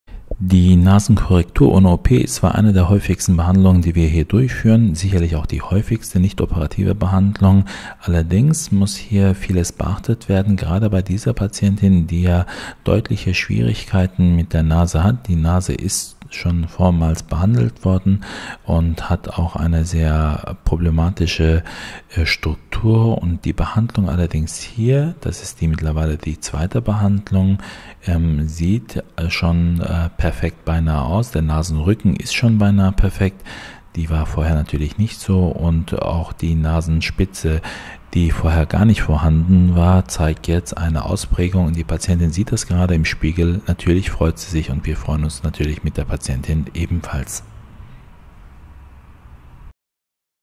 Mc Aesthetics präsentiert die Nasenkorrektur ohne OP
Eleonora B. aus Stuttgart schreibt:,,Ich bedanke mich bei Mc Aesthetics. Mein Aufenthalt und die gesamte Betreung war einfach klasse. Der Service ist wirklich überdurchschnittlich hoch und ich bin auch noch dazu sehr zufrieden mit meinem Ergebnis. Die Nasenkorrektur war genau das richtige für mich.